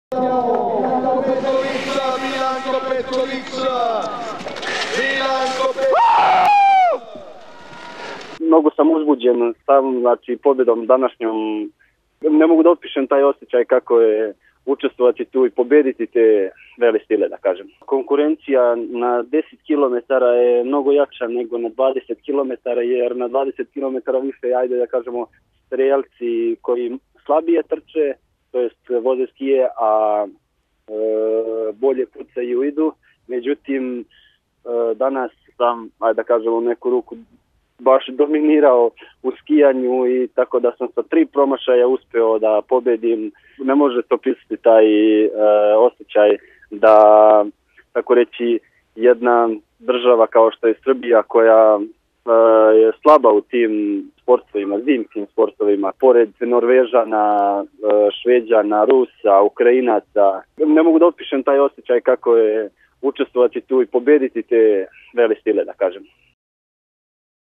Petroviću zlato na Univerzijadi
Srpski bijatlonac Milanko Petrović osvojio je prvu zlatnu medalju za Srbiju u istoriji na Univerzijadi u Trentinu! Copyright : Javni Servis Srbije (RTS)